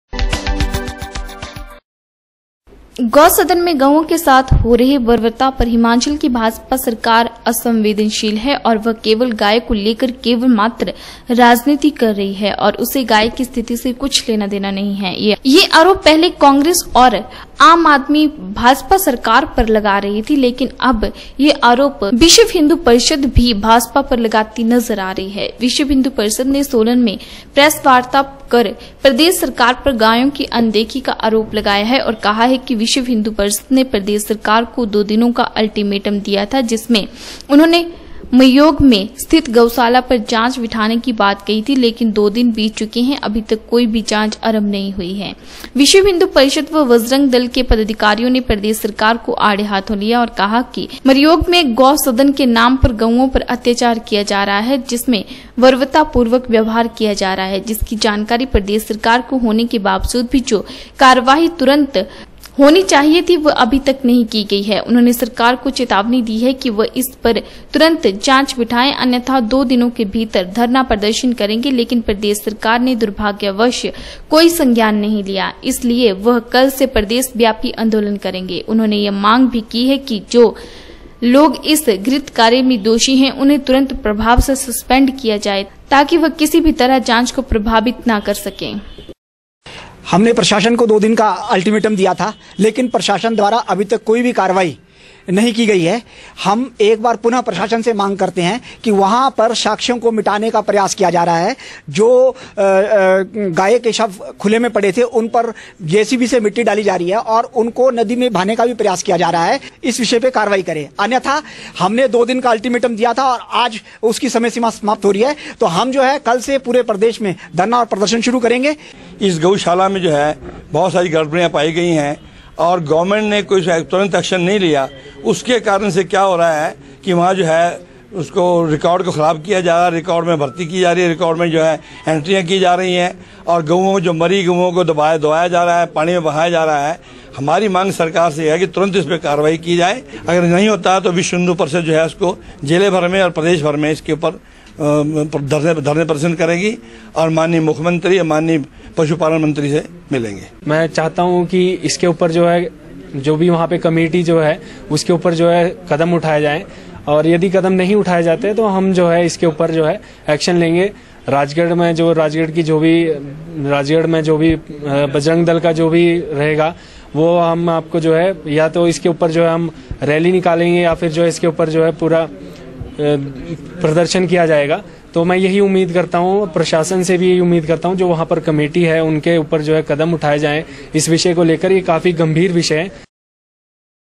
गौ सदन में गायों के साथ हो रही बर्बरता पर हिमाचल की भाजपा सरकार केवल राजनीति कर रही
गौ सदन में गायों के साथ हो रही बर्बरता पर हिमाचल की भाजपा सरकार असंवेदन शील है और वह केवल गाय को लेकर केवल राजनीति कर रही है और उसे गाय की स्थिति से कोई लेना देना नहीं है द्य यह आरोप पहले कांग्रेस और आम आदमी भाजपा सरकार लगा रही थी लेकिन अब यह आरोप विश्वहिंदू परिषद ही भाजपा पर लगाती नज़र आ रही है द्य विश्वहिंदू परिषद ने सोलन में प्रेस वार्ता कर प्रदेश सरकार पर गायों की अनदेखी का आरोप लगाया है और कहा कि विश्वहिन्दू परिषद ने प्रदेश सरकार को दो दिनों का अल्टीमेटम दिया था जिसमे उन्होंने मरयोग में स्थित गौशाला पर जांच बिठाने की बात कही थी लेकिन दो दिन बीत चुके है अभी तक कोई जांच आरम्भ नहीं हुई है